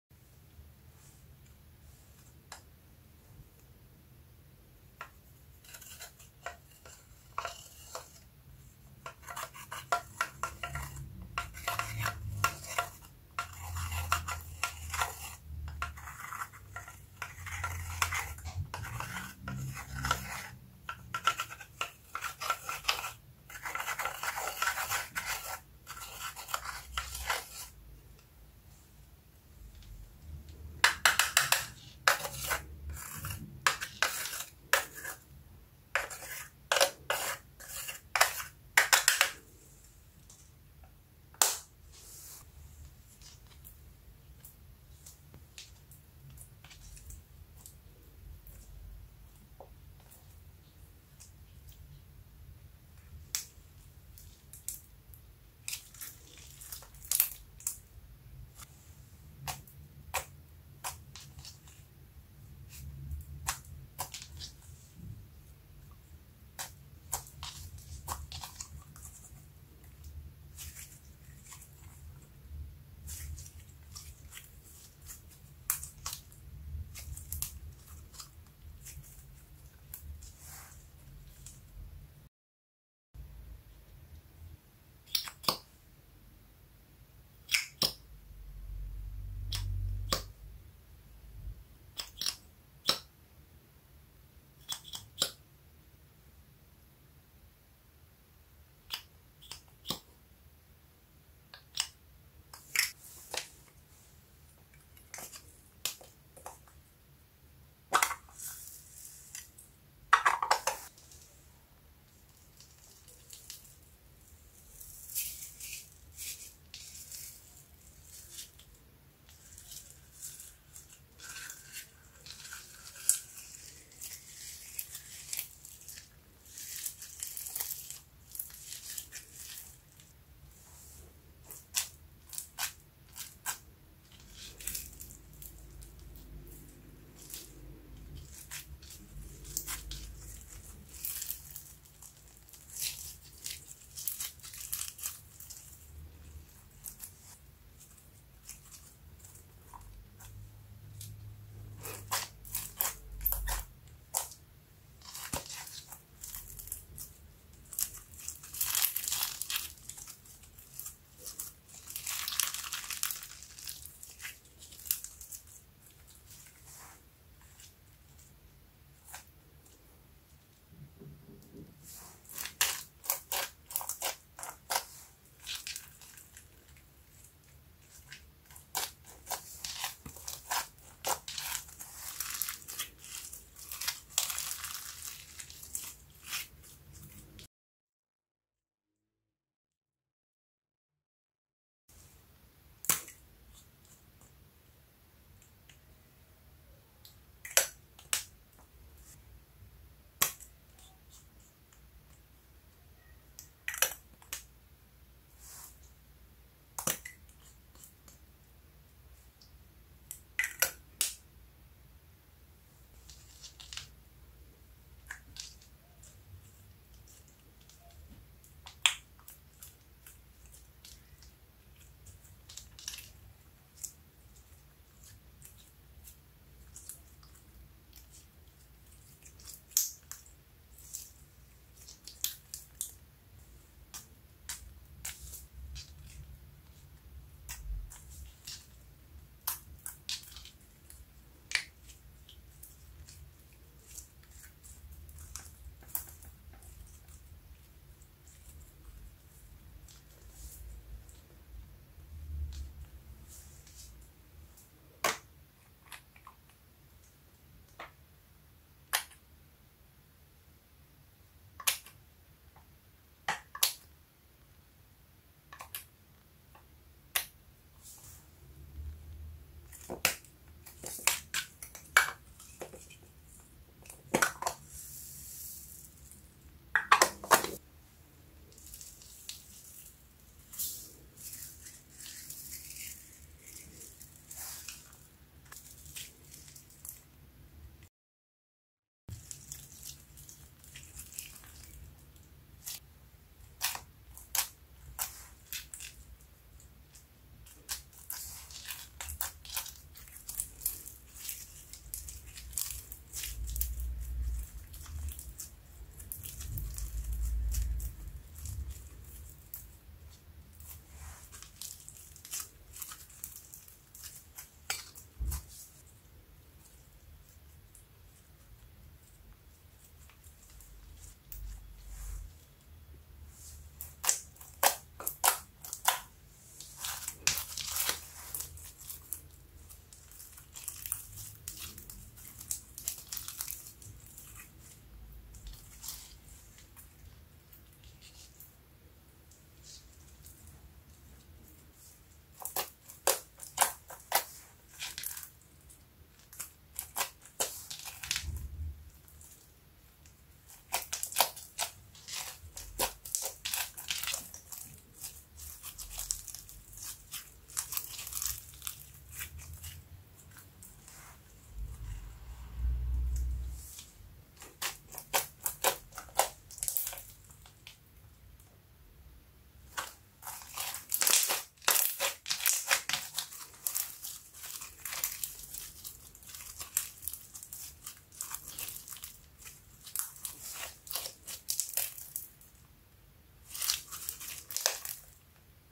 ASMR - Mixing Makeup slime coloring. glitter, Lipstick eyeShadow Makeup into Slime. Satisfying Slime
Slime Coloring with Makeup Compilation ! Most Satisfying Slime ASMR Videos #76 - SaddleBrown vs White vs DeepPink vs LightSkyBlue
メイクアップ編集によるスライムカラーリング [音フェチ]